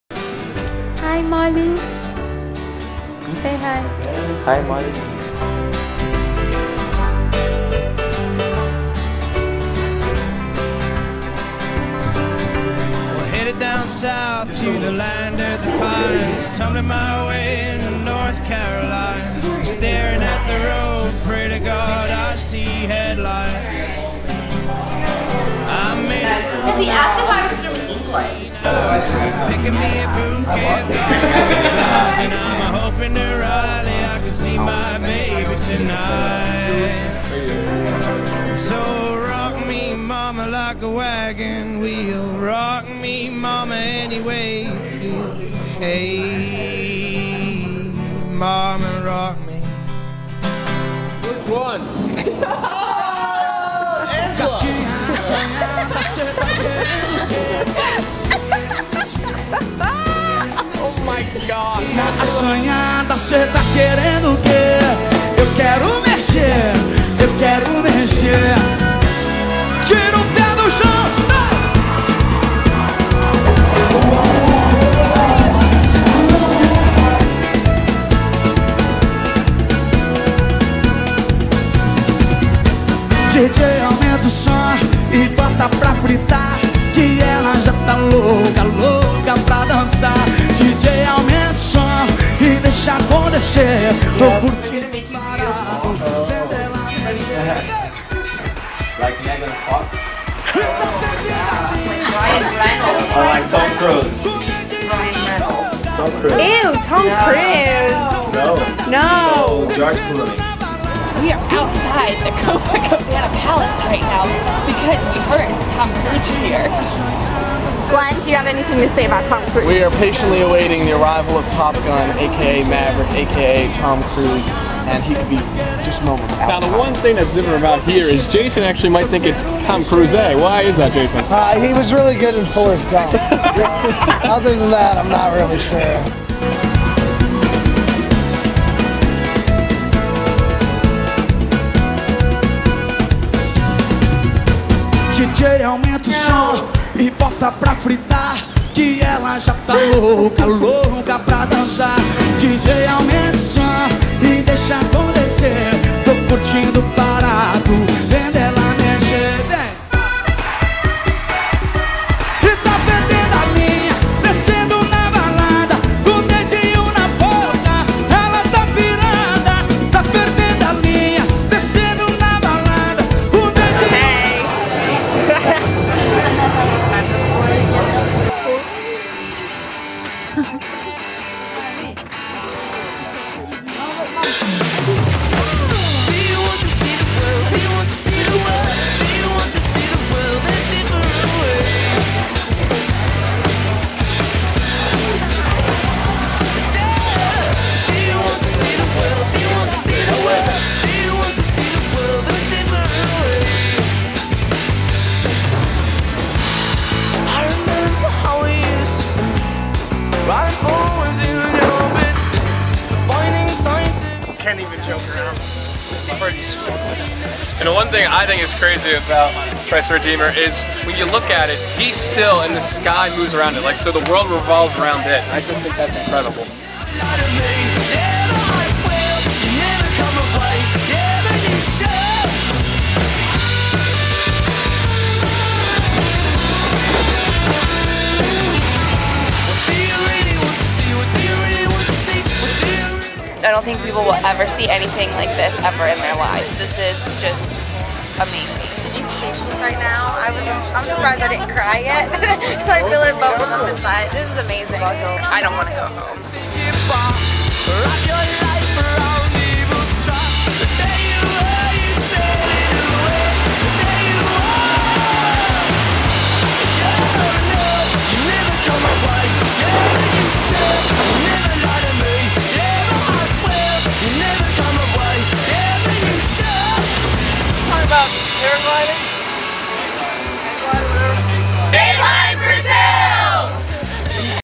Coconuts and Yahoos
So, Lin Yutang once said that "no one realizes how beautiful it is to travel until he comes home and rests his head on his old, familiar pillow." I think Mister Yutang is correct.

I was sad to leave Brazil, but I was also ready to come home, see my family and my friends and finish off the final days of my senior year. Now that I'm back in the states and the reality of who and what I left behind has set in... I cried putting together this video - reminiscing on the trip and the people who completely changed my life. I wish you could have come home with us.

The beauty behind Brazil exists in the people we met, the friends we made, the sights we saw and the memories we will have forever.

Obrigada, Brazil. XOXO.